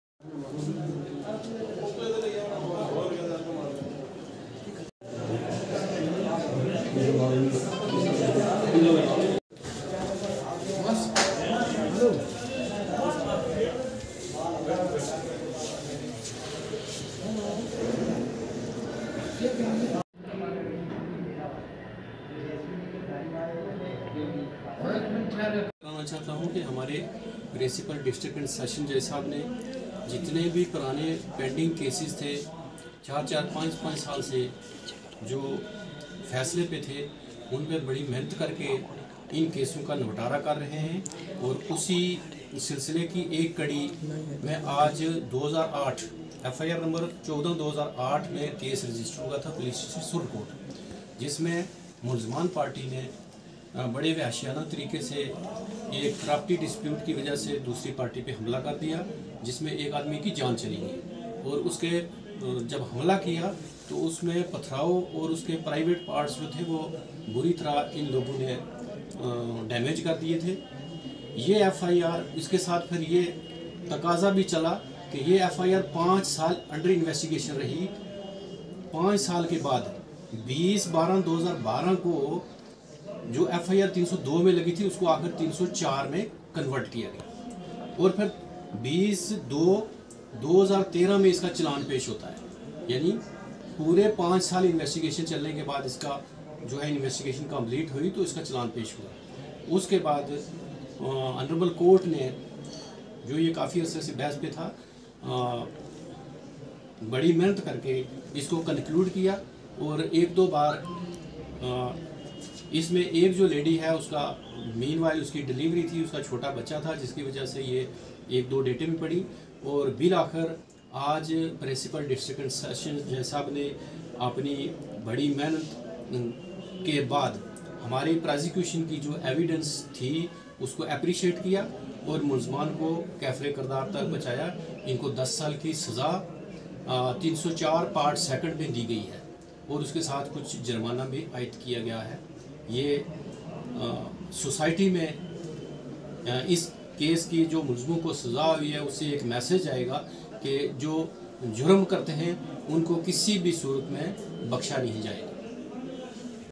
Learned Principal District and Sessions Judge Poonch Sh. Ashok Kumar Shavan has awarded
Learned Principal District and Sessions Judge Poonch Sh. Ashok Kumar Shavan has awarded 10 years imprisonment and fine of rupees 5000 for offence U/S 304-Conviction in offences U/S 304-II/RPC
II/RPC to four accused persons namely (1) Roshan Din S/O Hassan Mohd(2) Nazira Bi W/O Roshan Din (3) Sona BI W/O Hakam Din and (4) Safia Bi W/O Nazam Din all residents of village Hari Tehsil Surankote in case FIR No 14/2008 of P/S Surankote. As per prosecution story supra mentioned FIR was registered on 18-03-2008 on the written complaint of one Mohd Bashir S/O Mohd Hassan R/O Hari Tehsil Surankote Wherein he alleged that on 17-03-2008 at about 4:PM all the accused persons with their criminal intention have made murderous attack on the bother of complainant namely Mohd Sharief S/O Mohd Hussain while he was busy in construction work of his house on his own land under Khasra No. 373. During murderous attack accused No 01 Roshan Din hit him with the stone on his head due to which the brother of complainant fell down while as the other accused No 04 namely Nazia Bi also hit him with the stone on the abdomen of the said victim. The victim Mohd Sharief brother of the complainant died on spot due to murderous attack upon him by the accused persons. Investigation carried on by different I.Os for five years in the offence U/S 302/109/RPC and finally on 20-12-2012 offence U/S 302 was dropped and offence 304-II was inserted against the accused persons. Charge sheet was filed on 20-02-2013 in the Court of law. Today the Hon’ble Court held all the accused persons guilty and awarded ten years imprisonment and fine to the tune of Rs5000/- each with respect to accused No 01 and 02 whereas Accused No 03 & 04 have to undergo simple imprisonment of 10 years. Prosecution conducted through Mr. Maqbool Hussain Public Prosecutor for U.T and Ld. Advocate Shokat Amin Manhas appeared on behalf of accused persons